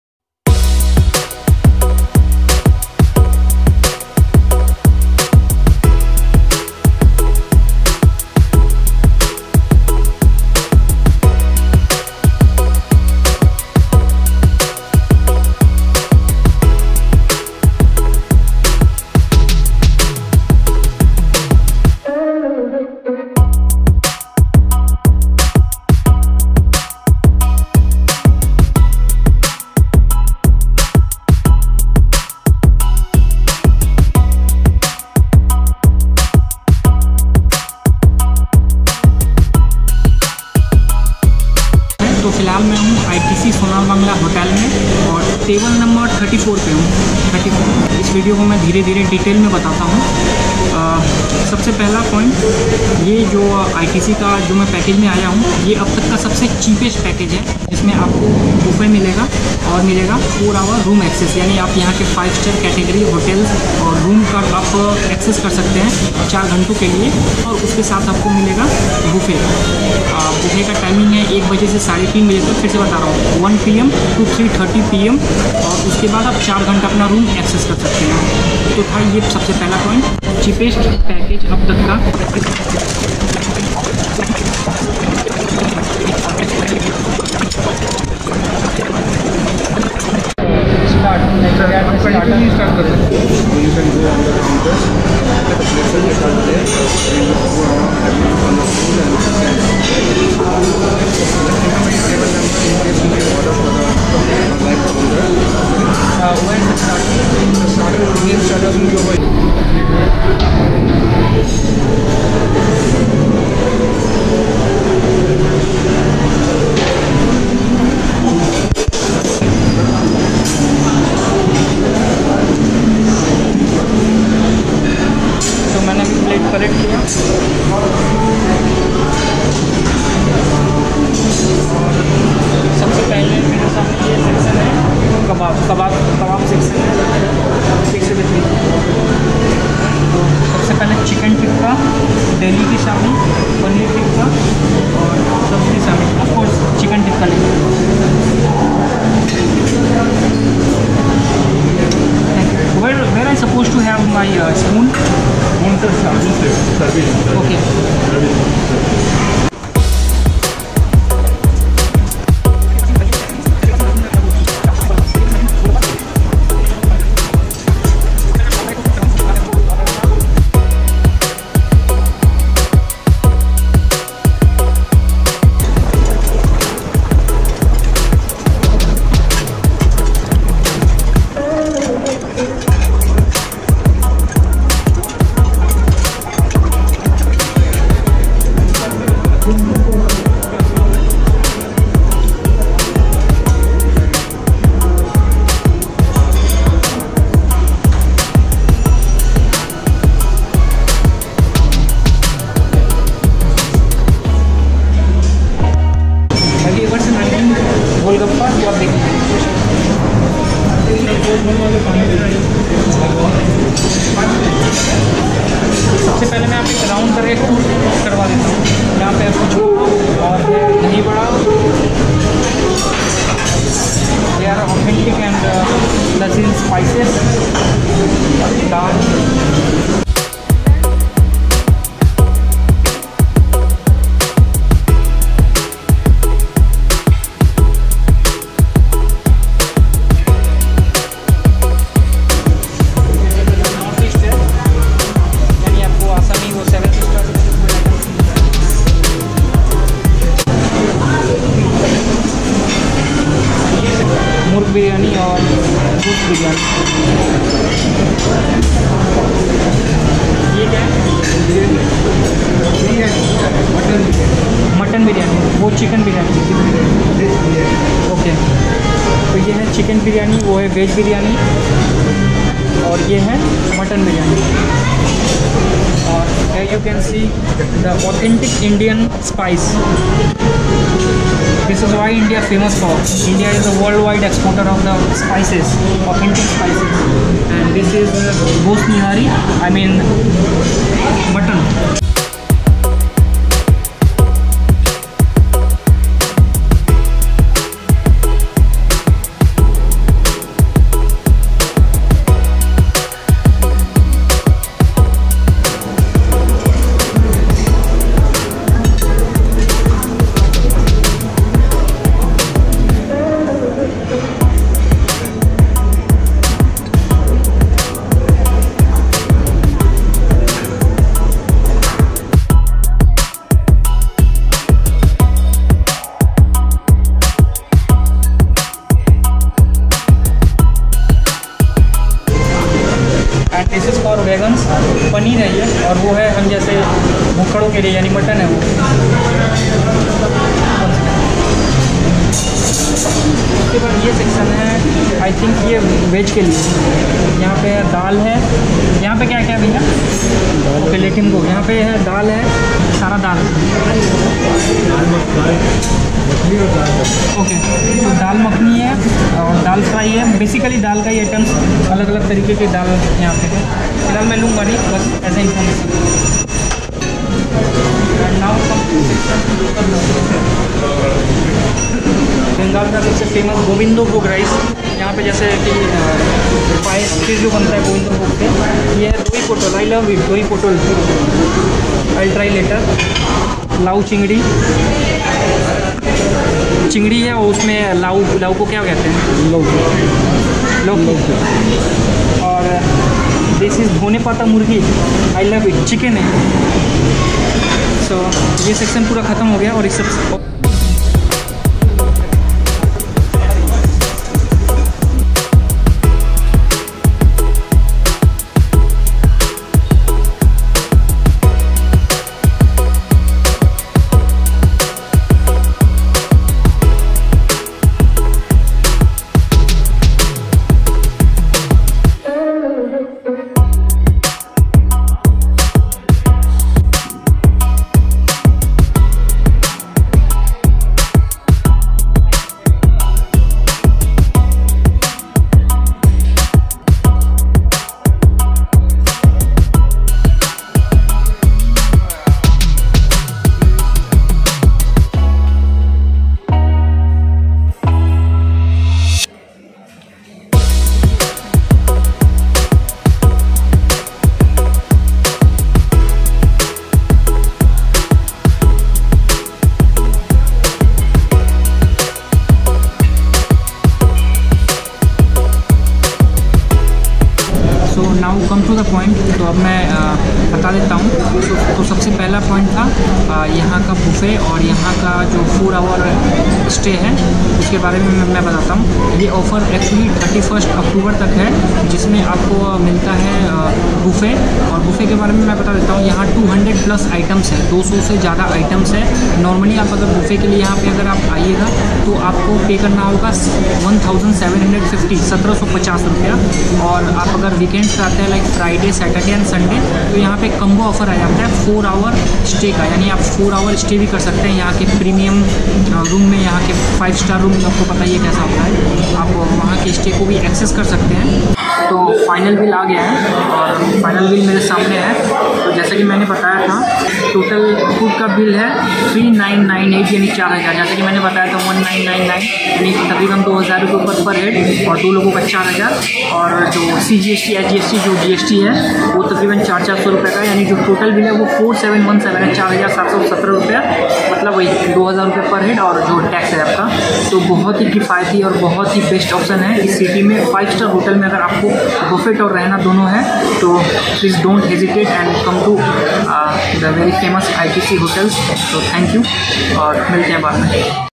Hotel Sonar Bangla | 5 star Cheapest buffet || ITC Royal Bengal
A day in a Most Luxurious 5 star | Hotel Sonar Bangla | 5 star buffet || ITC Royal Bengal
In this video I introduce you to a buffet tour at ITC Royal Bengal. This is about how much it will cost to have a lunch at ITC Royal Bengal, and how many menus are served over there. The most special thing is that they are offering a combo pack of Unlimited Buffet + 4 hr room access at only 4717rs only. So enjoy it.